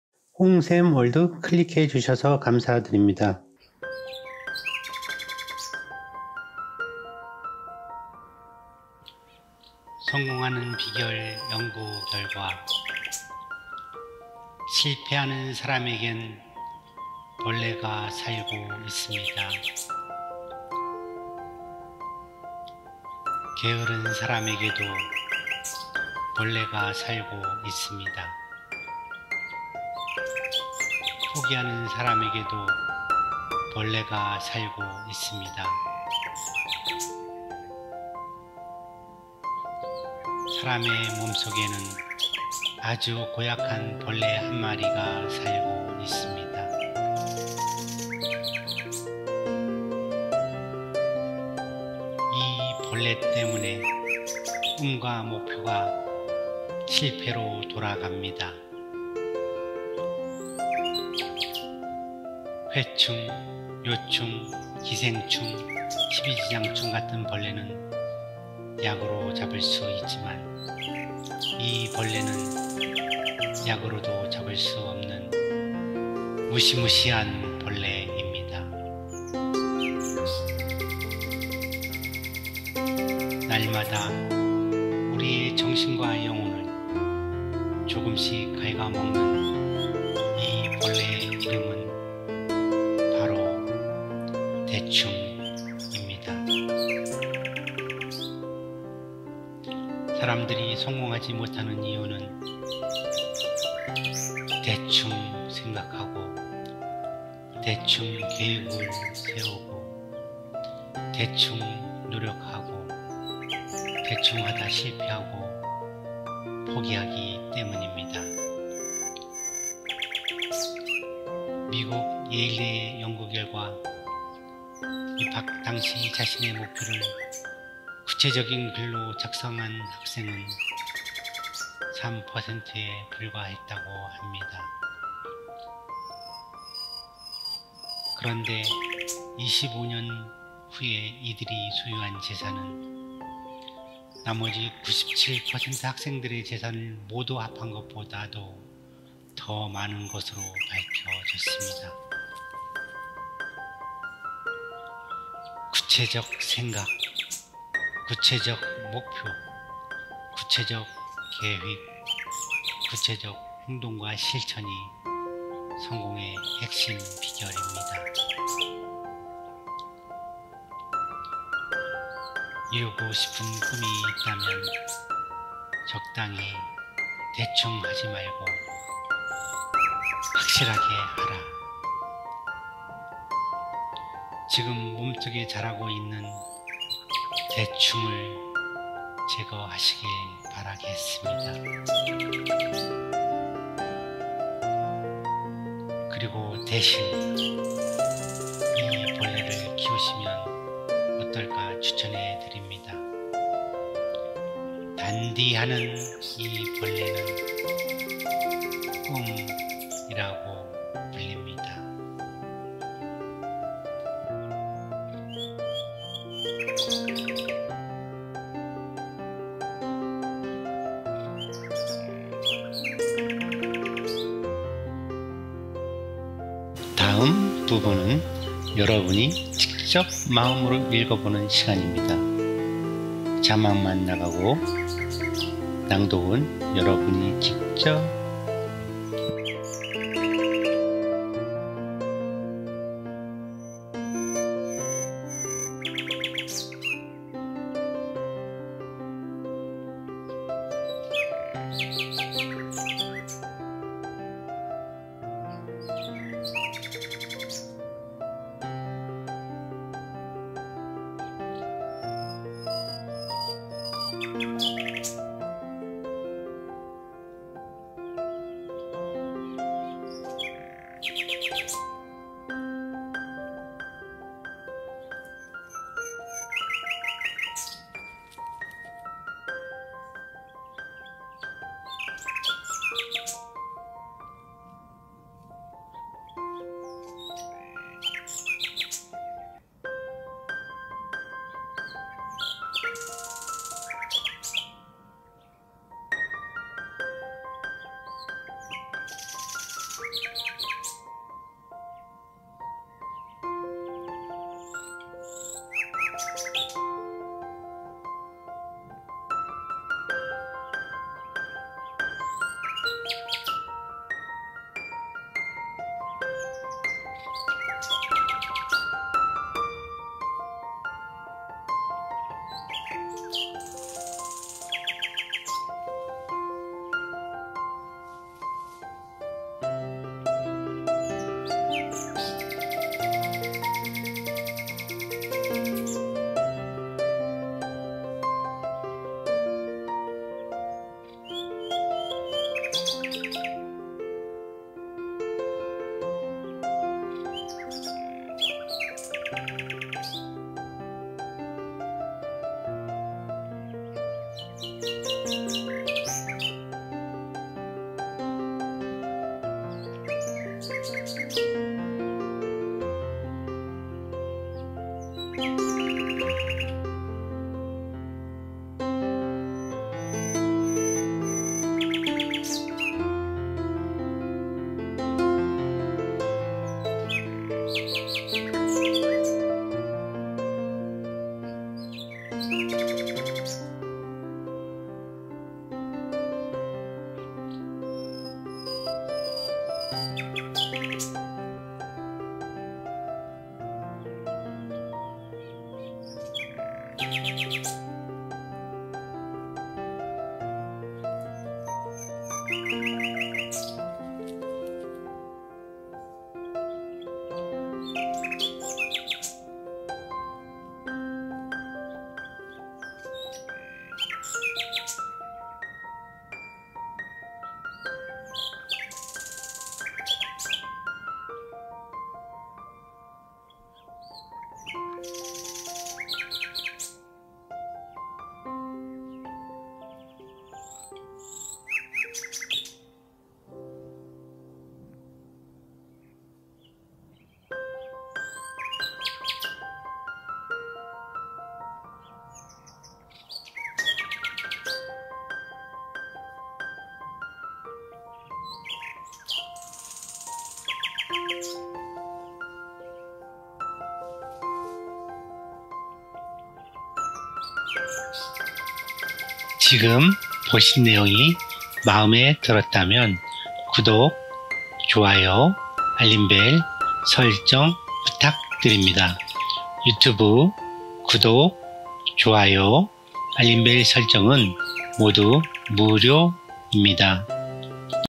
좋은글, + 성공하는 비결 연구결과 / 미 예일대, 구체적 행동과 실천이 성공의 핵심비결입니다
좋은글
미 예일대 성공하는 비결 연구결과
실패하는 사람에겐 대충이라는 벌레가 살고 있습니다.
구체적 행동과 실천이 성공의 핵심비결입니다

+ 성공하는 비결 연구결과 / 미 예일대

실패하는 사람에겐
벌레가 살고 있습니다.

 회충, 요충, 기생충,
십이지장충 같은 벌레는
약으로 잡을 수 있지만,
이 벌레는 약으로도 잡을 수 없는
무시무시한 벌레입니다.

날마다 우리의 정신과 영혼을
조금씩 갉아 먹는 이 벌레의 이름은
바로 "대충"입니다!

구체적 생각,
구체적 목표,
구체적 계획,
구체적 행동과 실천이
성공의 핵심비결입니다.

그리고 대신 이 벌레를 키우시면
어떨까 추천해 드립니다.
단디 하는 이 벌레는~
"꿈"이라고 불립니다.